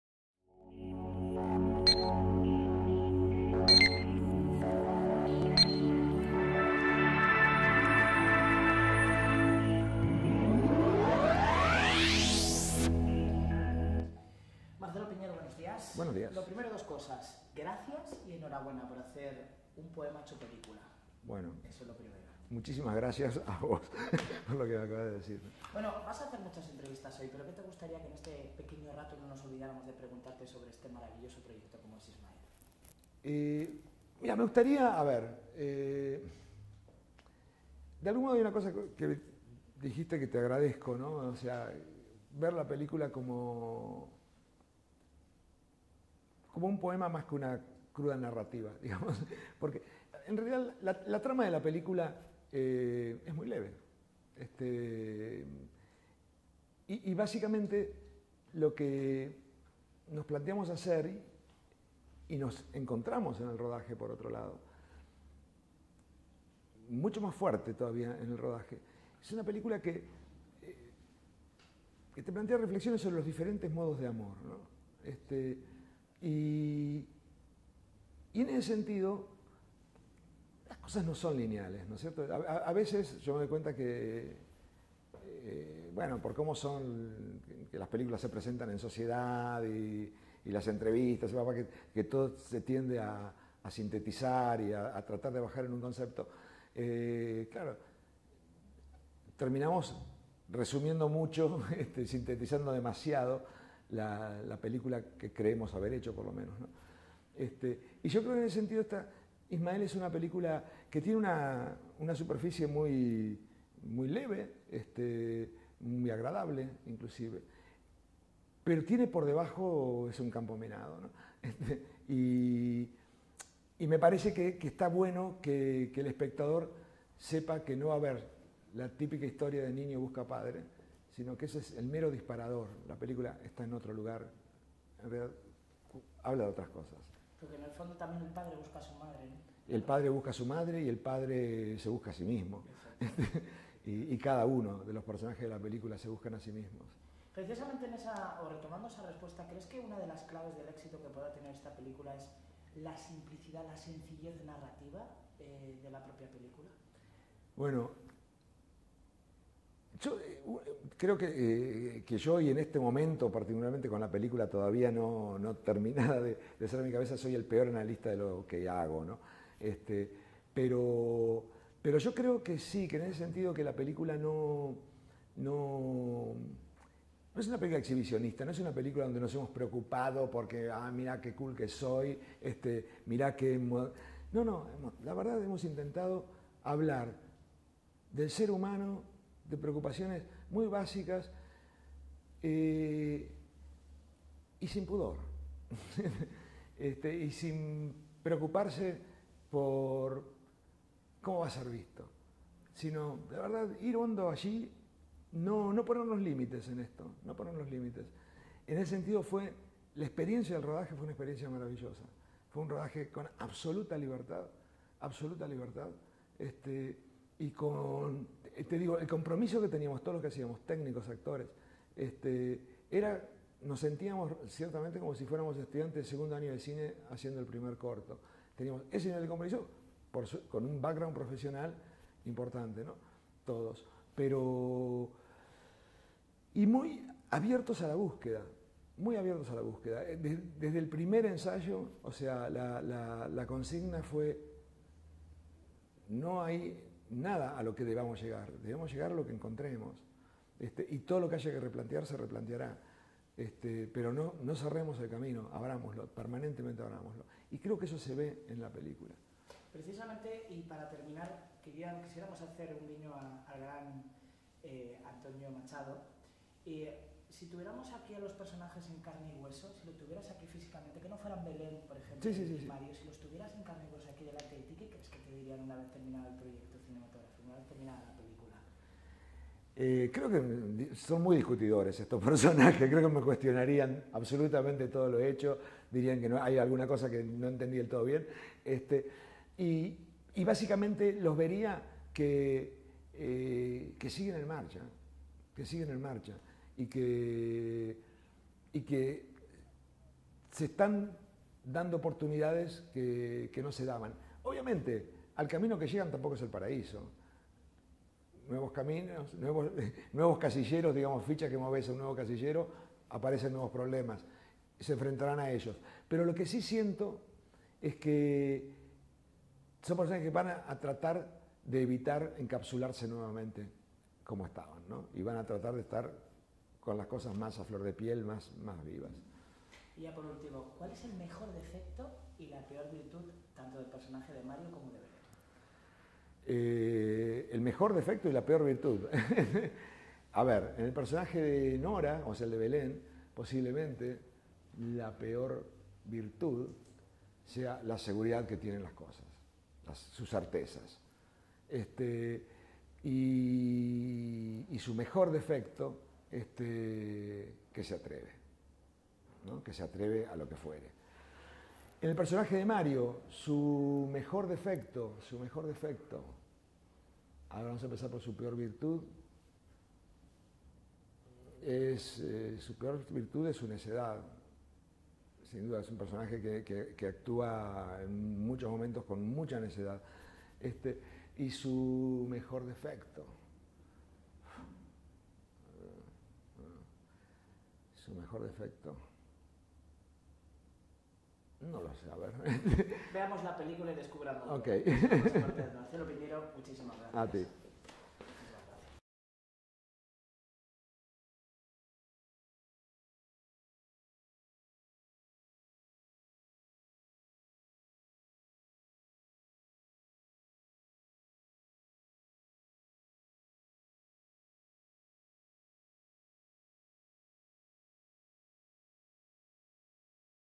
LACAJADMUSICA.ORG | MARCELO PIÑEYRO - ISMAEL | DICIEMBRE 2013
Mario Casas , Belén Rueda , Marcelo Piñeyro visitaron "La Caja de Música" (lacajadmusica.org) para presentarnos la que dicen es la mejor película del año 2013, "Ismael" de Marcelo Piñeyro. Nos contaron todos los secretos de la película en una entrevista muy especial .
Idea original : David Fraile
Imágenes : Lacajadmúsica TV